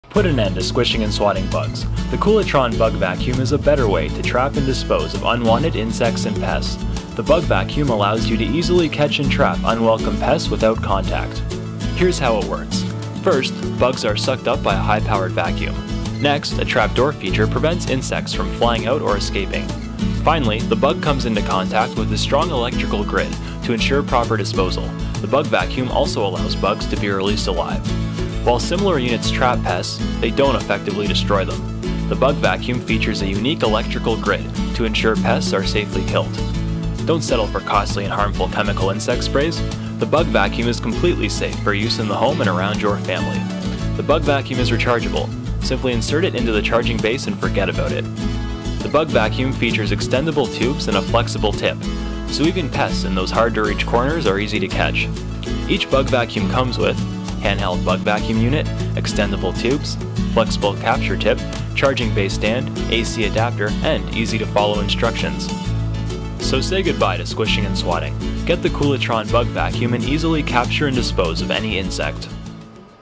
Bug Vacuum by Koolatron
No more chasing bugs around your home! Bug Vacuum creates a stylish, high-powered suction that captures unwanted pests. A trap door feature securely traps captures pest, insects within the nozzle and prevents them from flying back out. It is a safe, efficient and non-toxic alternative to costly, toxic bug sprays in your home. The Bug Vacuum is an easy, efficient and non-toxic way to control bugs, insects and other pests that fly or crawl.